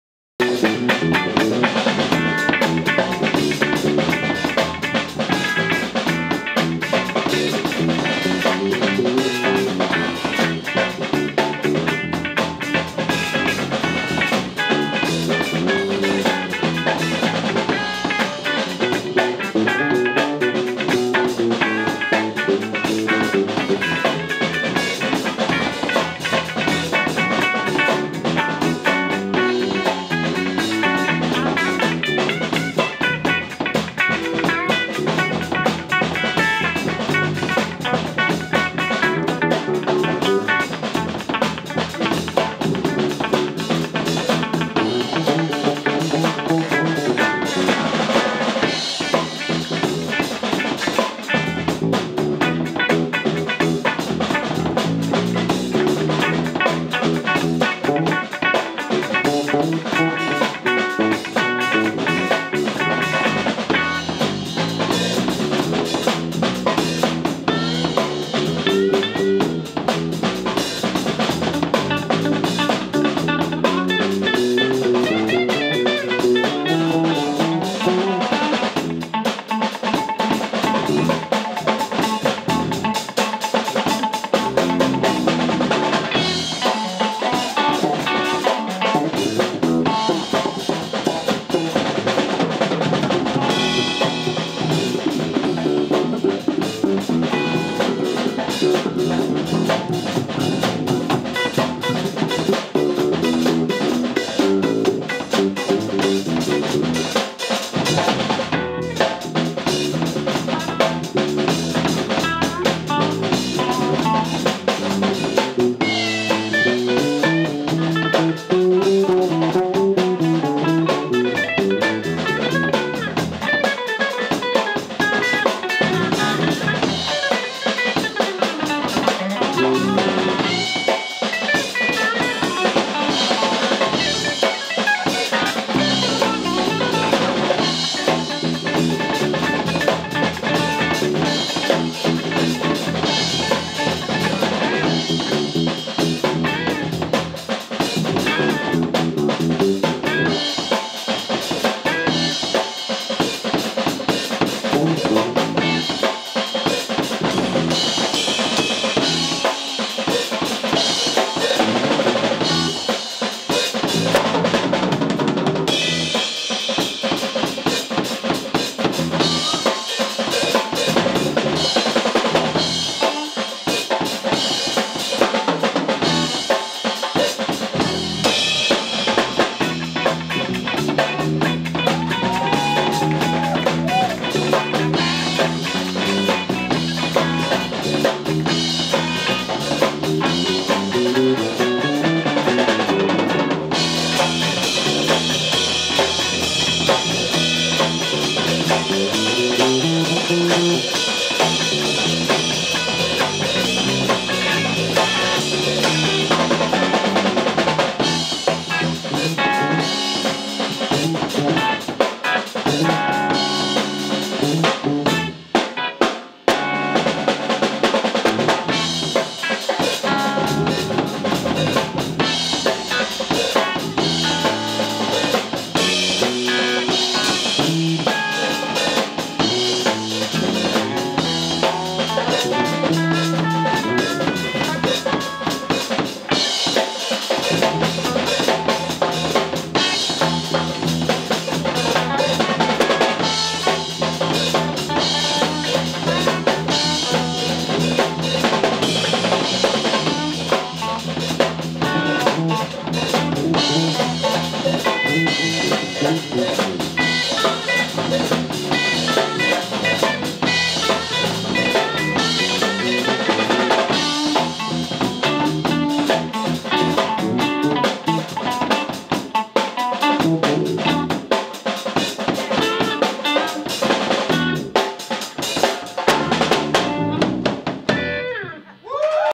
BAASSIK at Brooklyn Blues open mic at Bush Baby, Brooklyn, NY (2013)
Thanks to Brooklyn Blues, here is an impromptu jam at Bush Baby in Brooklyn from 2013. Totally on-the-spot and on-the-fly, BAASSIK leads with Gabriel Chaplan (guitar) and Lorenzo Boston(drums).